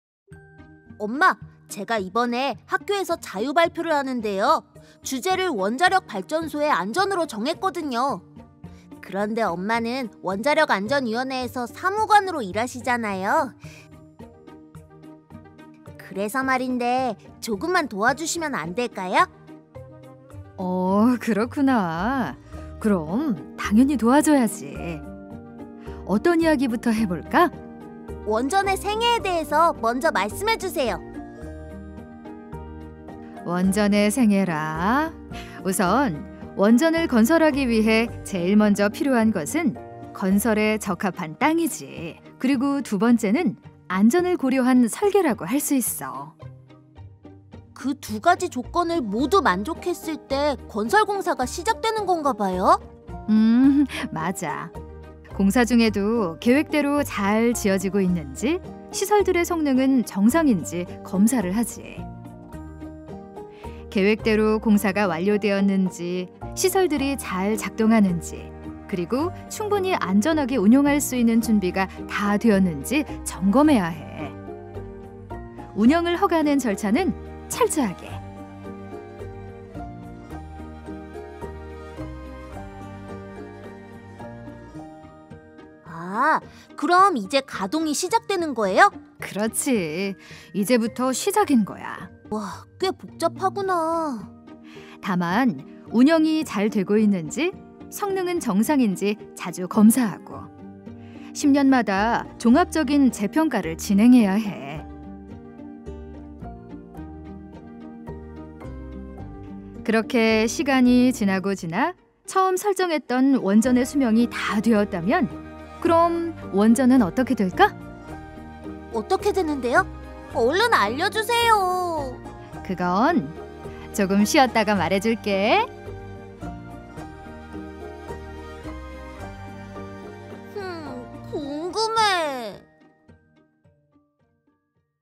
[원안위-툰] 1화. 건설 허가에서 안정성 평가까지
[원안위-툰]1화. 건설 허가에서 안정성 평가까지 '원전의 생애'를 알기 쉽게 웹툰으로 4편을 걸쳐 제작하였으니 많은 관심 부탁드립니다:)